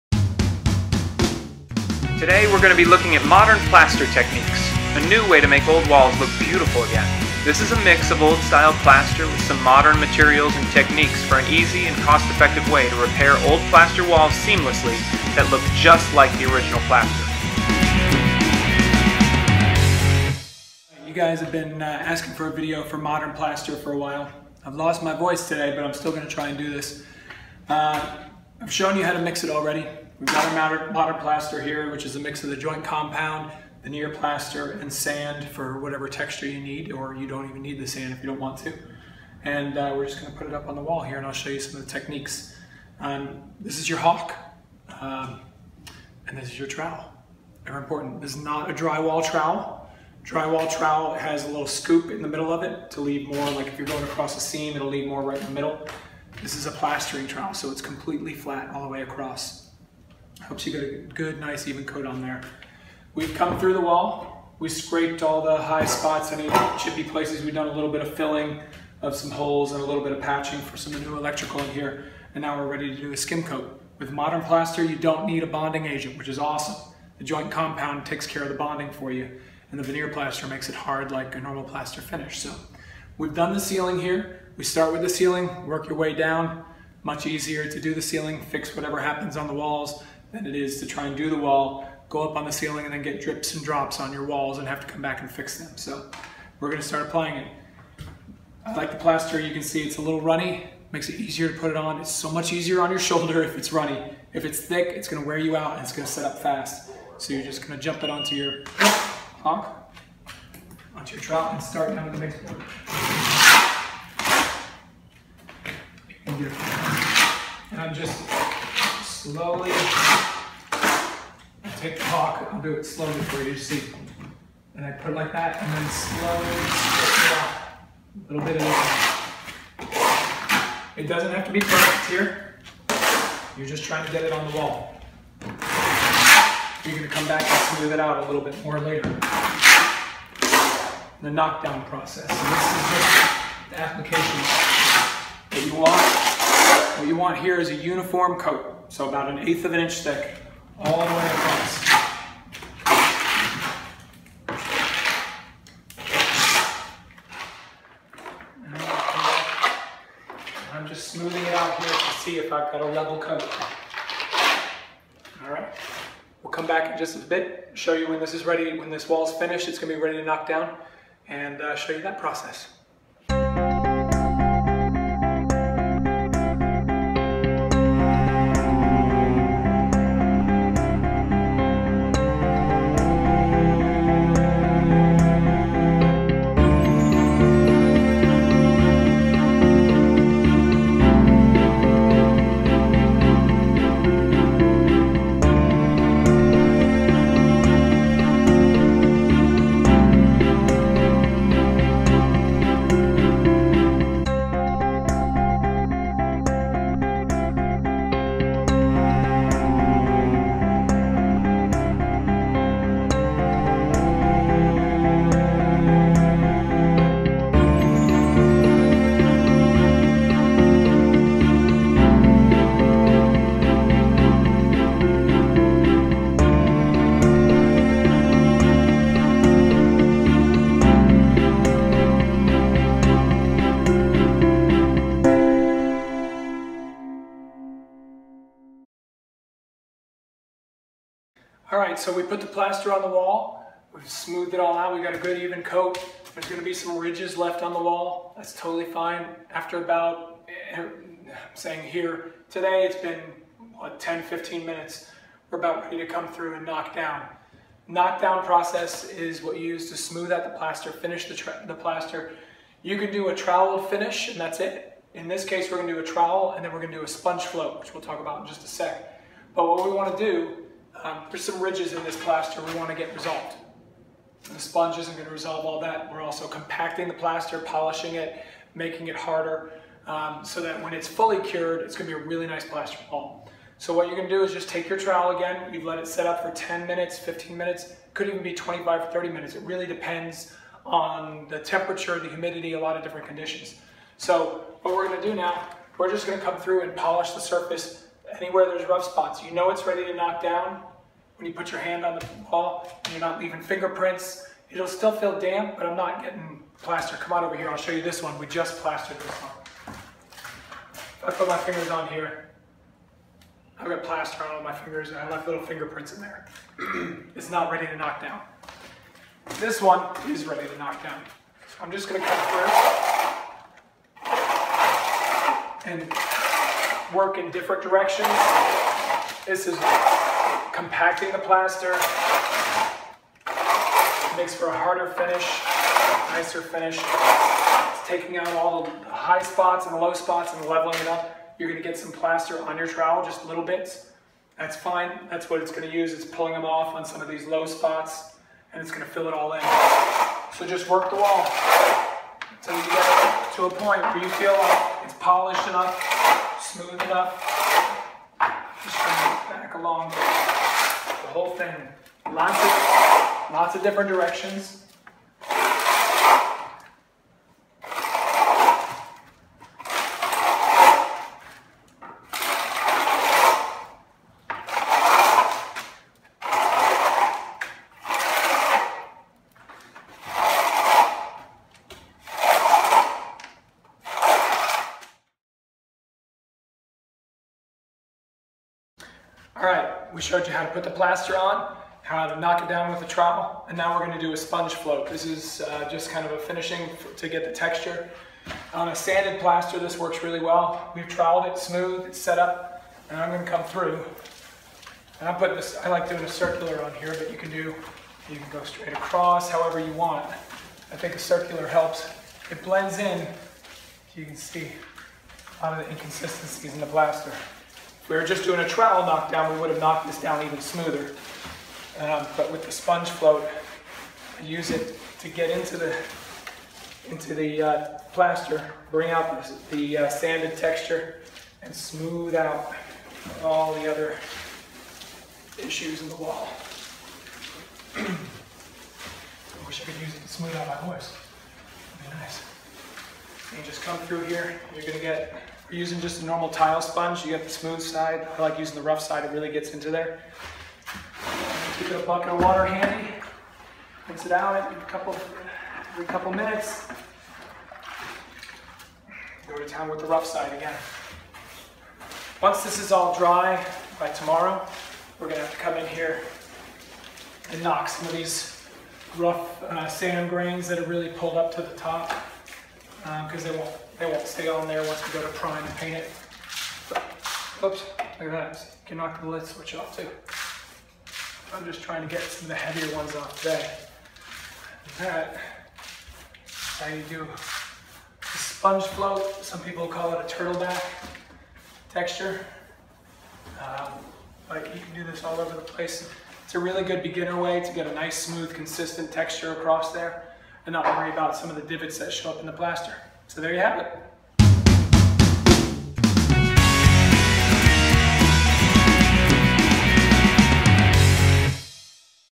Modern Plaster Techniques
Learn some easier techniques for repairing historic plaster walls and ceilings and how to make standard drywall look like historic plaster in the short video.